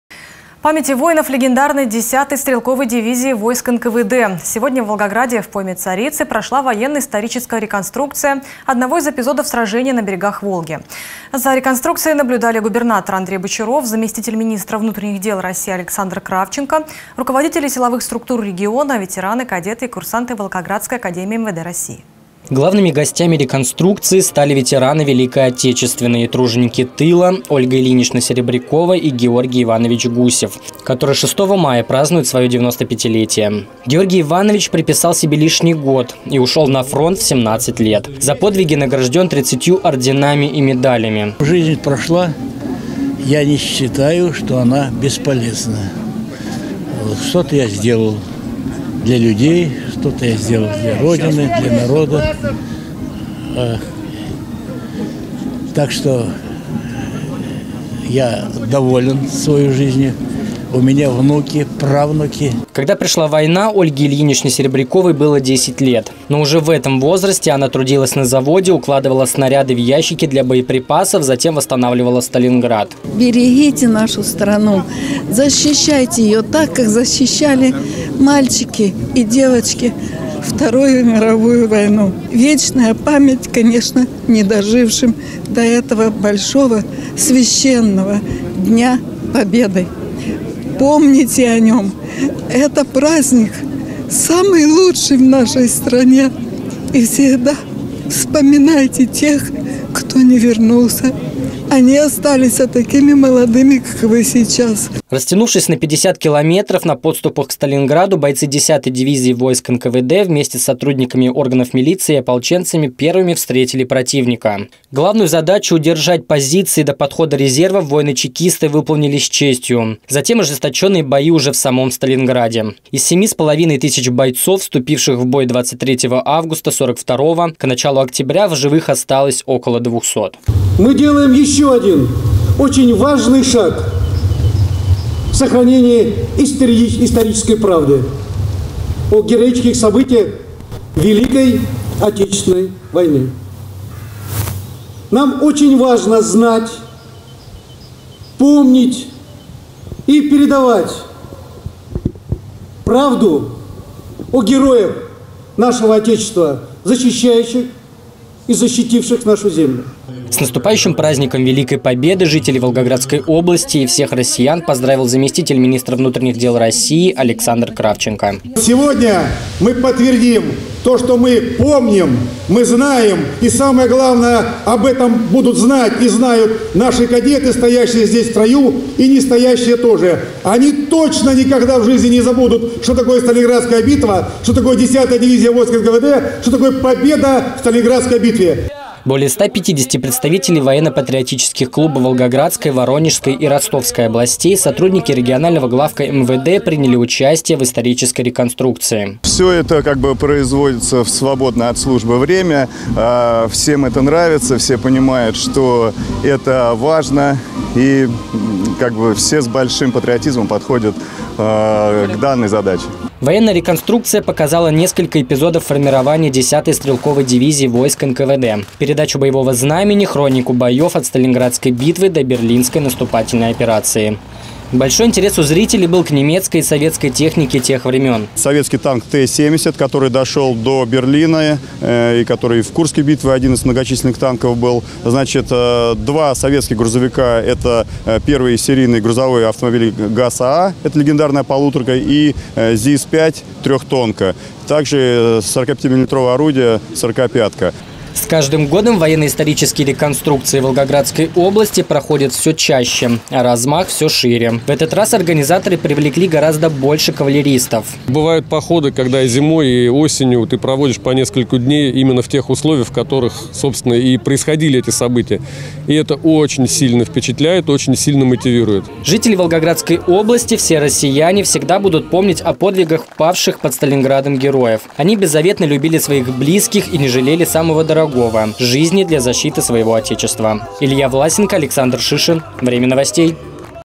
В Волгограде прошла военно-историческая реконструкция "Они были первыми"
Памяти воинов легендарной 10-й стрелковой дивизии войск НКВД. Сегодня в преддверии 77-й годовщины Победы в Великой Отечественной войне в Волгограде в пойме Царицы прошла военно-историческая реконструкция одного из эпизодов сражения на берегах Волги. За исторической реконструкцией наблюдали губернатор Андрей Бочаров, заместитель министра внутренних дел России Александр Кравченко, руководители силовых структур региона, ветераны, кадеты, курсанты Волгоградской академии МВД России.